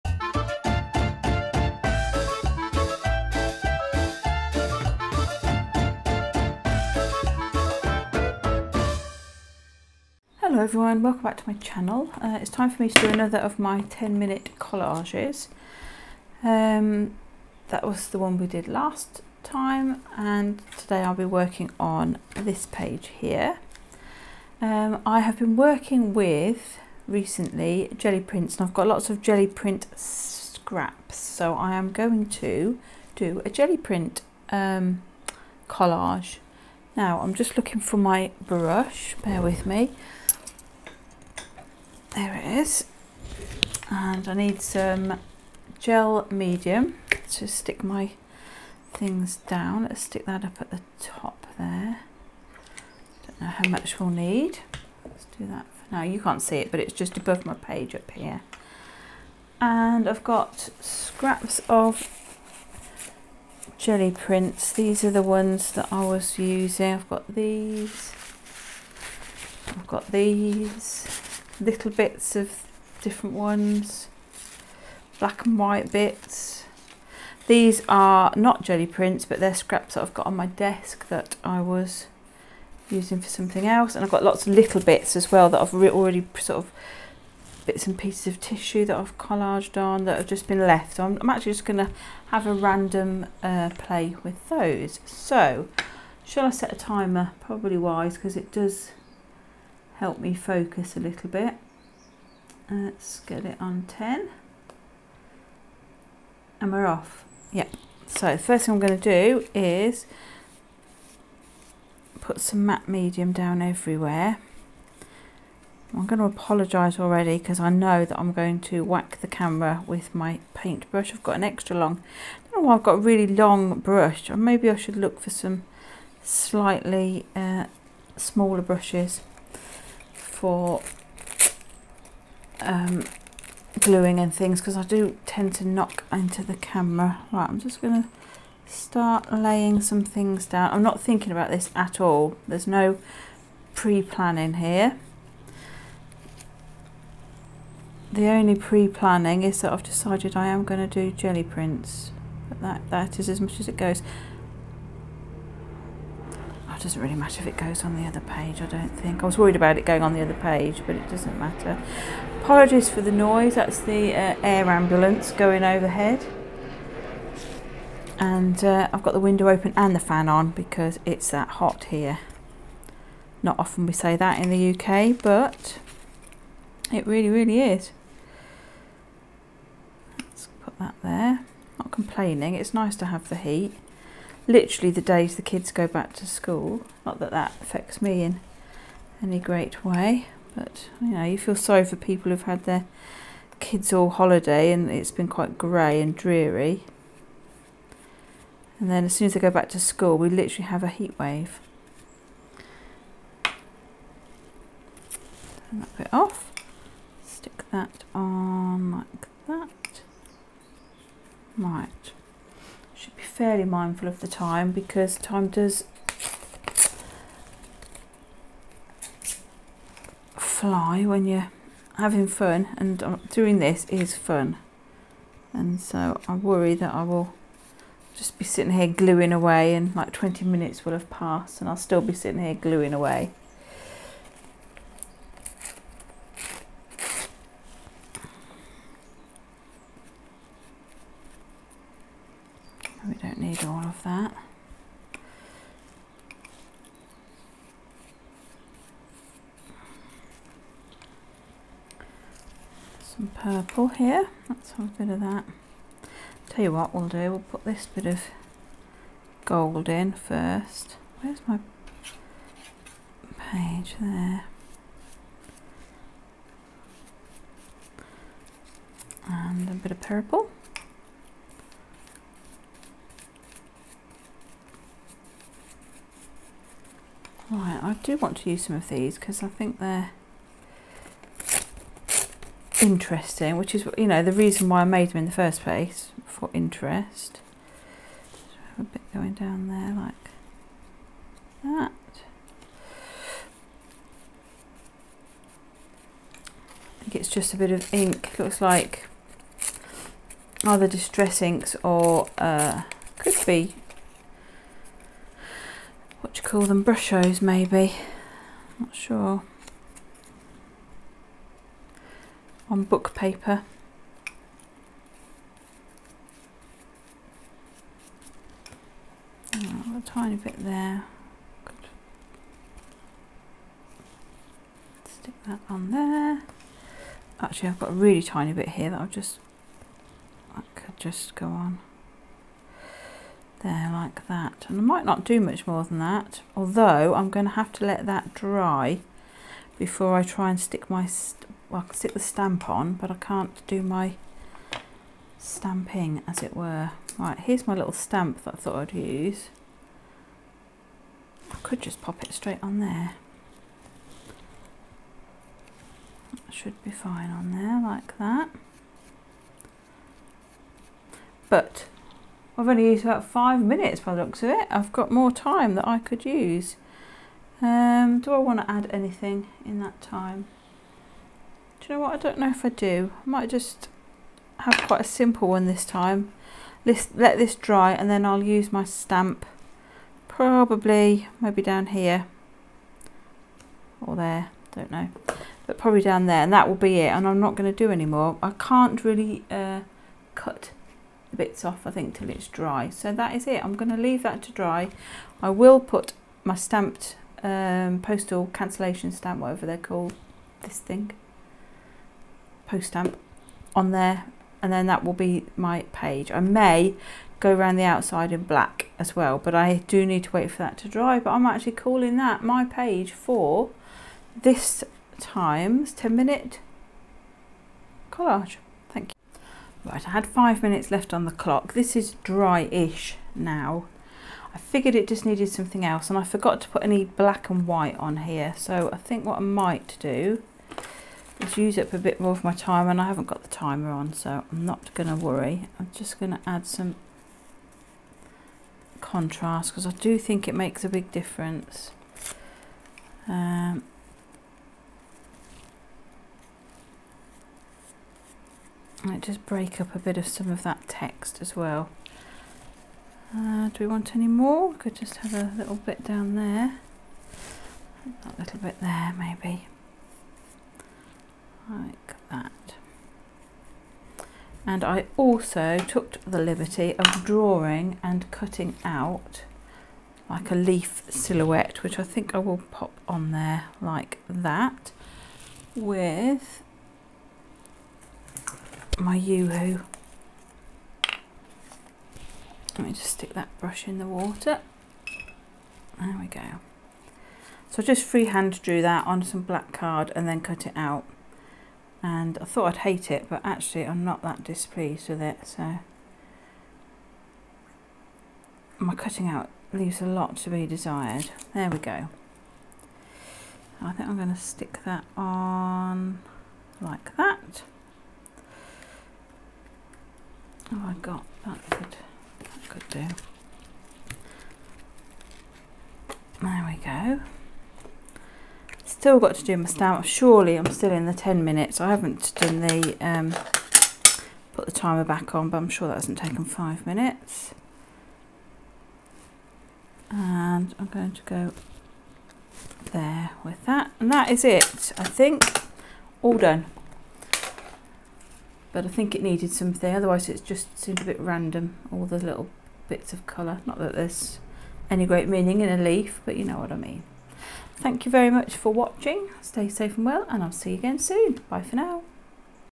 10MinuteCollage
Fancy using up all those fancy scraps you've got laying around on your craft desk? I have lots of things I save and never use. I'm going to create some quick collages using just scraps, stickers, glue and the odd embellishment. Why not join me? Hoping these will be intuitive and very quick to produce. Cheerful mindless crafting.